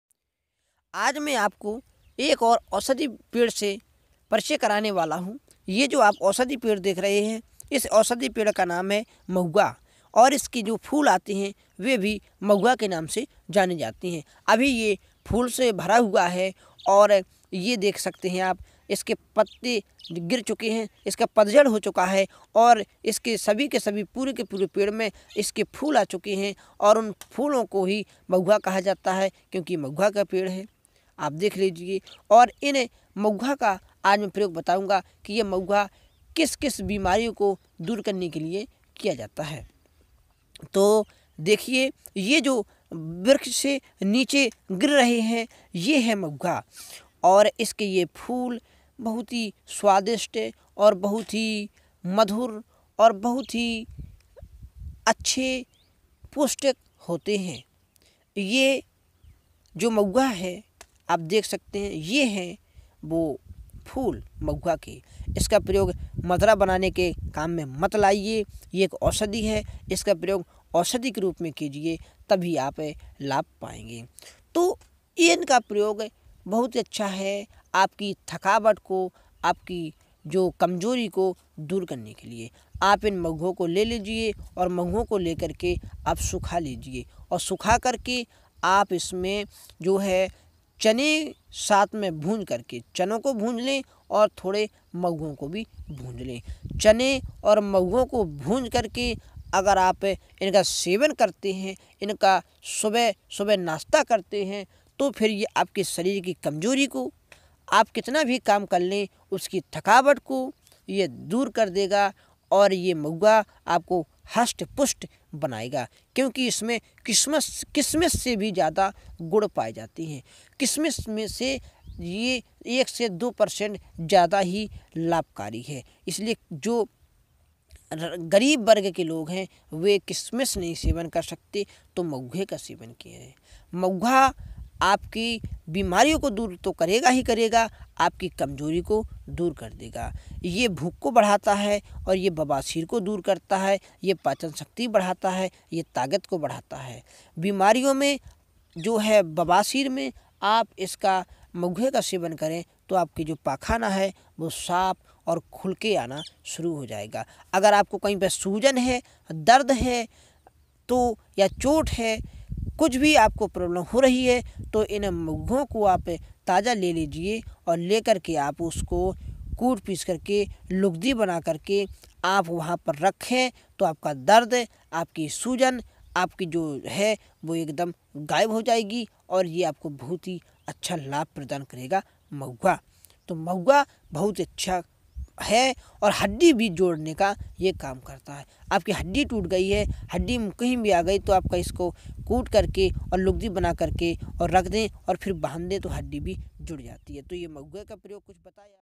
ये महुआ आपको कभी मरने नहीं देगा,आप हमेशा जवान बने रहोगे।
vedio के बारे में:
इस vedio में मैने आप सभी को बताया है कि कैसे आप इस औषधीय का प्रयोग करके अपने बीमारियों को खत्म कर सकते हो
इस vedio में  हमने आपको महुआ के कुछ चमत्कारी आयुर्वेदिक गुणों का वर्णन किया है।